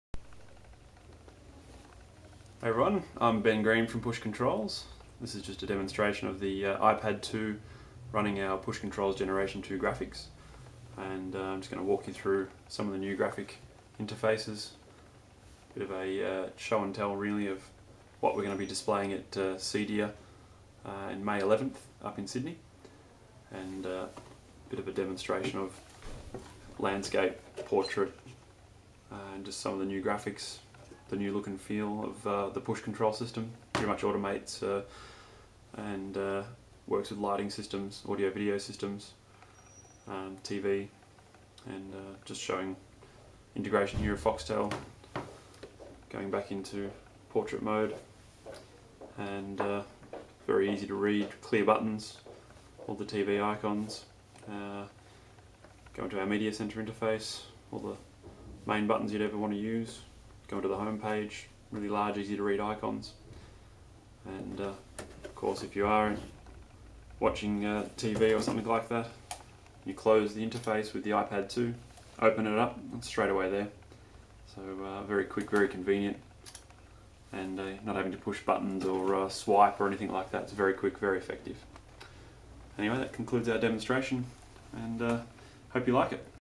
Push Controls Gen2 GUI
A preview of Push Gen2 graphics on an iPad2.

Gen2 will be launch by Push Controls at CEDIA, May 11th Sydney 2011.

Push is an affordable control solution that allows users to control their audio, visual, lighting, security and automation system from their iPad/iPhone/iTouch and Android device.